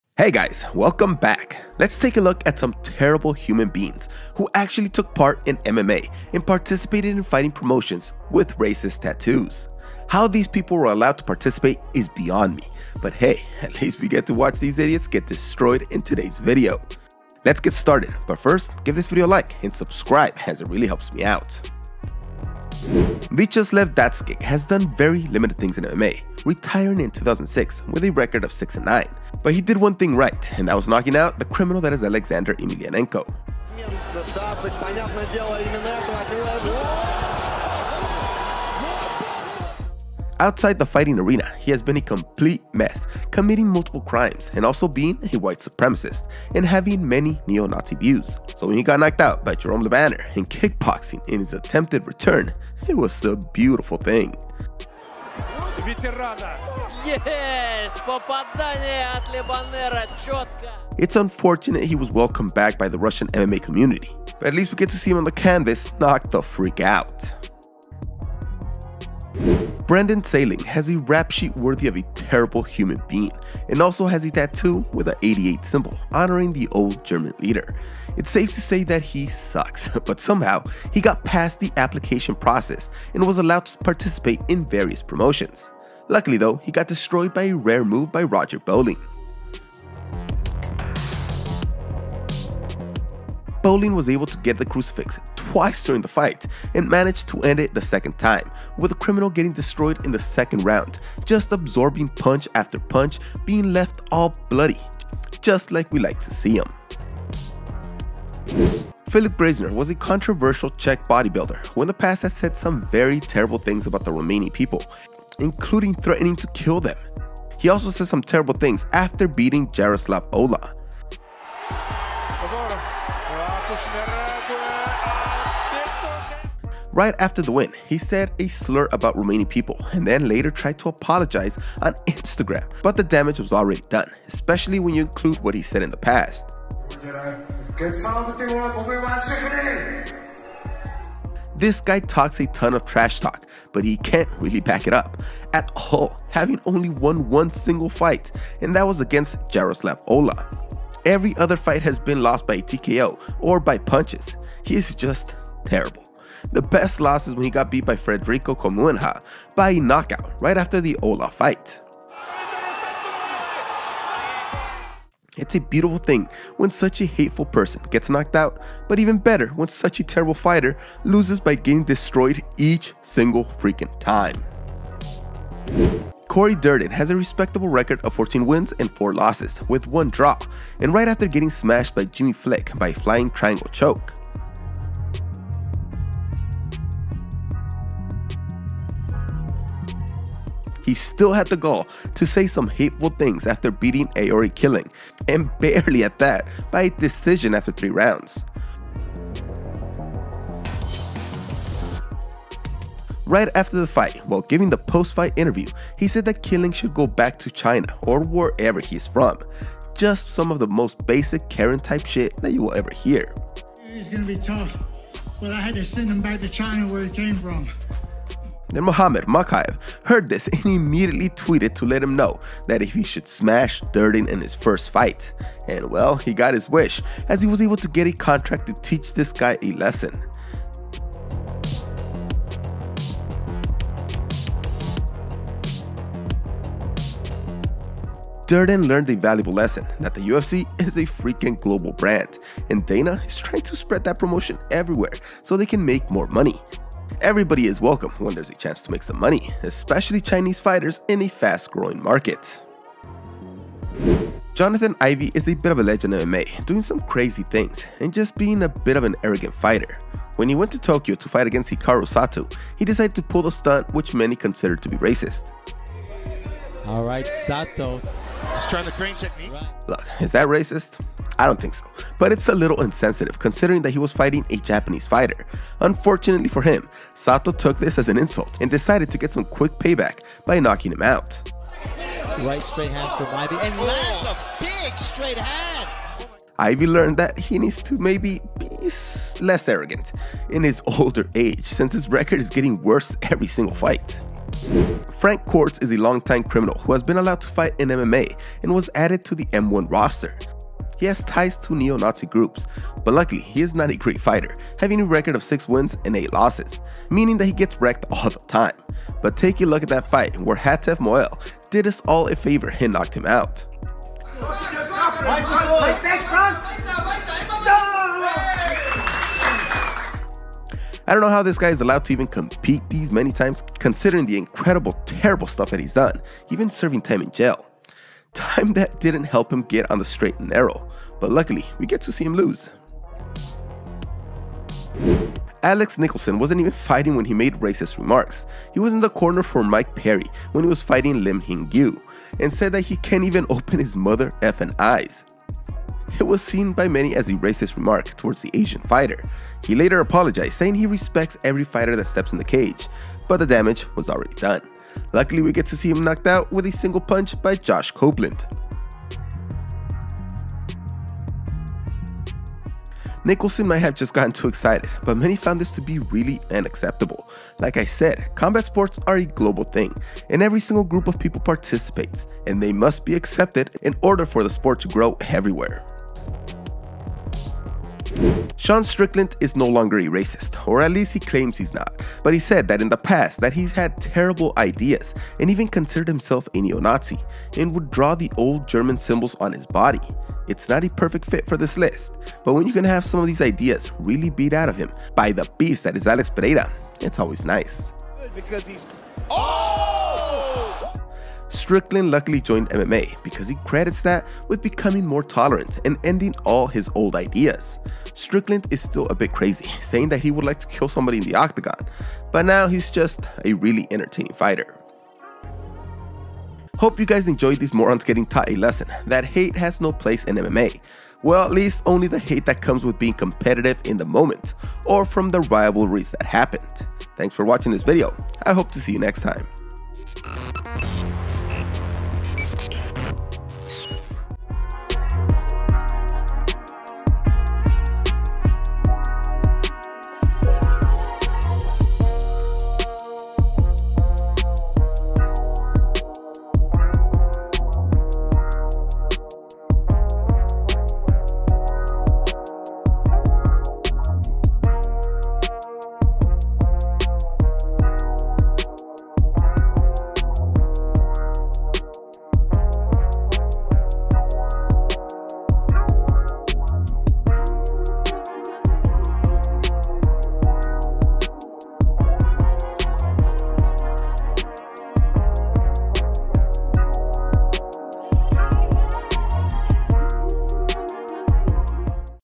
Top 10 Racist Fighters Getting Knocked Out in Professional MMA Fight
Professional MMA fighters who have racist views getting crushed.

top10, top 10, top 5, top5, mma, ufc, bellator, eagle fc, mixed martial arts, boxing, knock out, mma news

Editor: Sstudioarts - Instagram @sstudioarts25